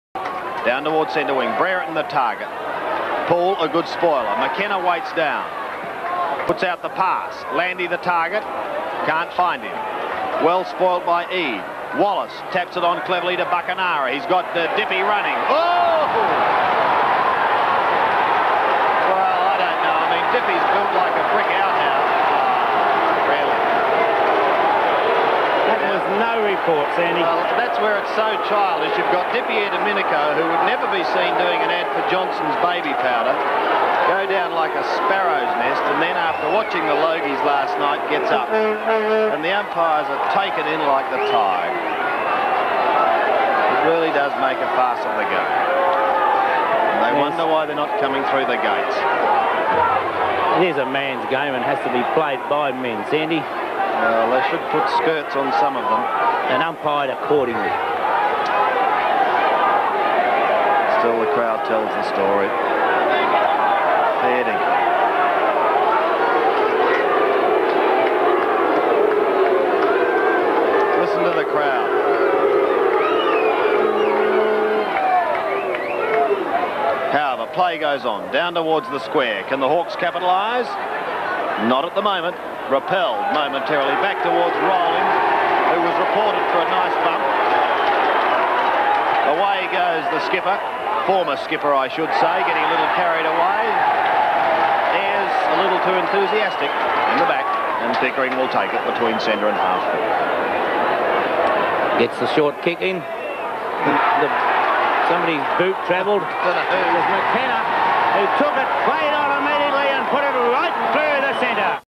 Barry Rowlings cleans up Big Dipper with a “nice bump” - 1985
Barry Rowlings cleans up Robert DiPierdomenico with a “nice bump”

1985 VFL Round 5
Hawthorn vs Richmond